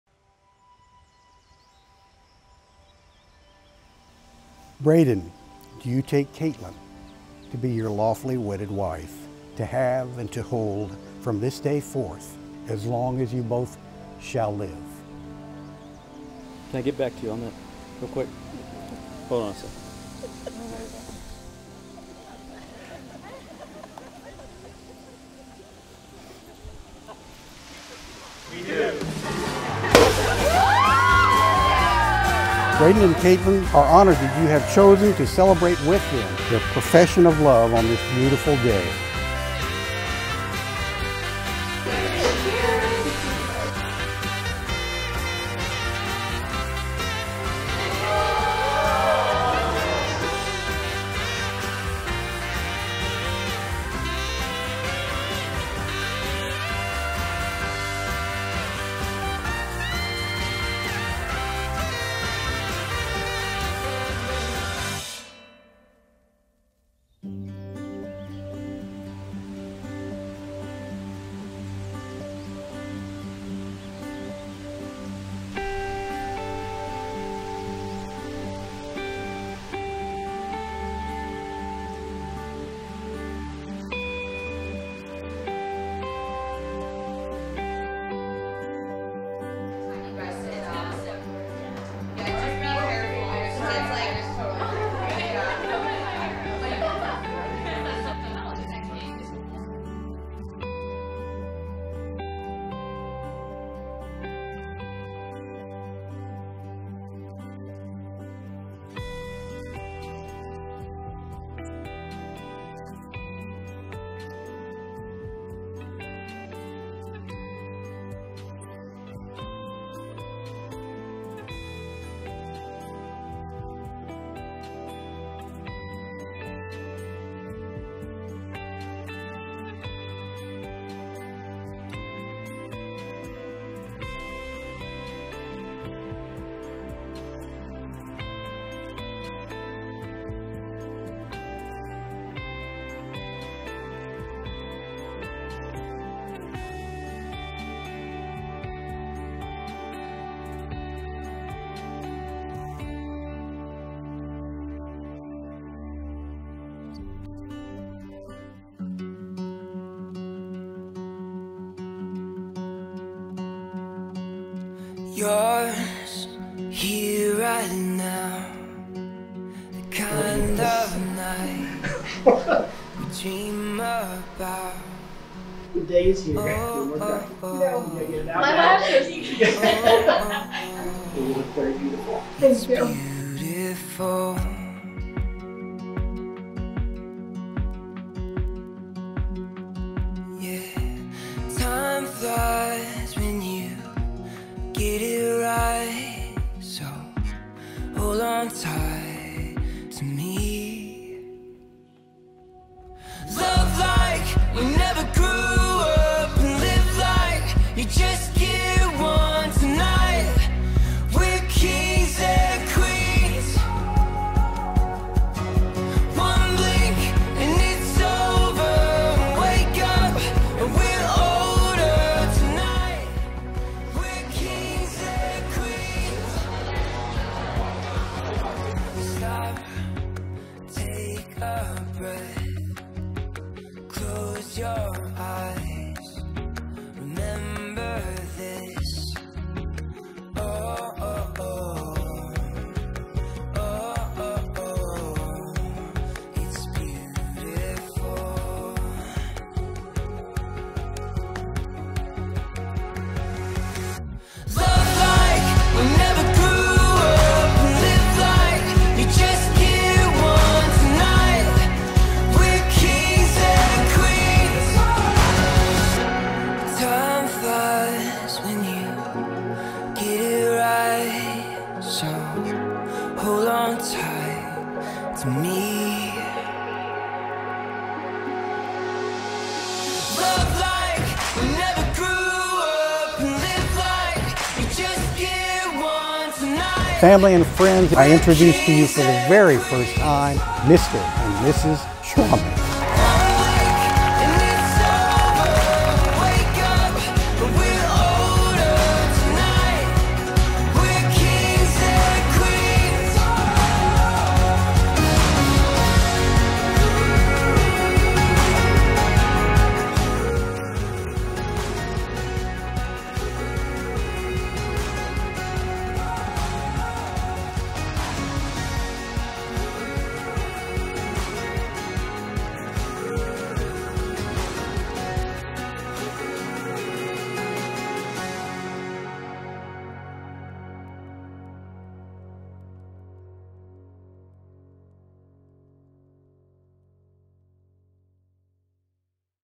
Braden & Katelyn | Wedding Film | Sunset Bluffs
We have shot many weddings in Missouri and in many different venue locations. This venue here has got to be one of my favorite venues! Braden and Katelyn pulled off an amazing wedding here in Washington, MO and we are just really glad to have been a part of their day!